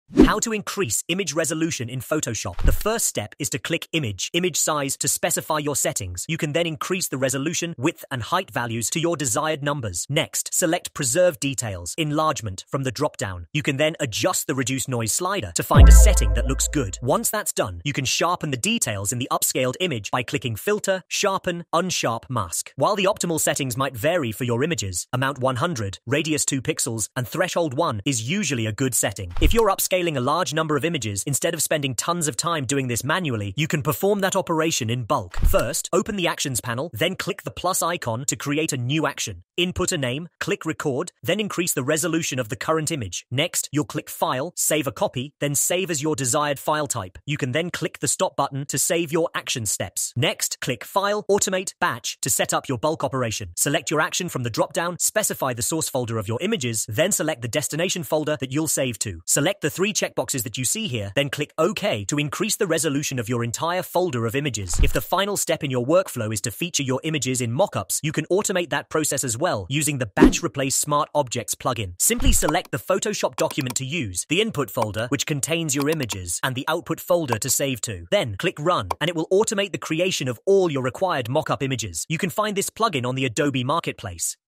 How To Increase Image Resolution In Photoshop
Boost the resolution of your images professionally with our Photoshop tutorial on how to increase image resolution without compromising quality. This guide is essential for photographers and designers who want to enhance photo quality for high-resolution outputs. Learn different methods to increase photo resolution in Photoshop, ensuring your images look their best whether displayed online or printed. From simple adjustments to more intricate techniques using Photoshop's powerful tools, this video will help you transform your images into high-quality assets with improved resolution and clarity.

Learn more about the Batch-Replace Smart Objects plugin for Photoshop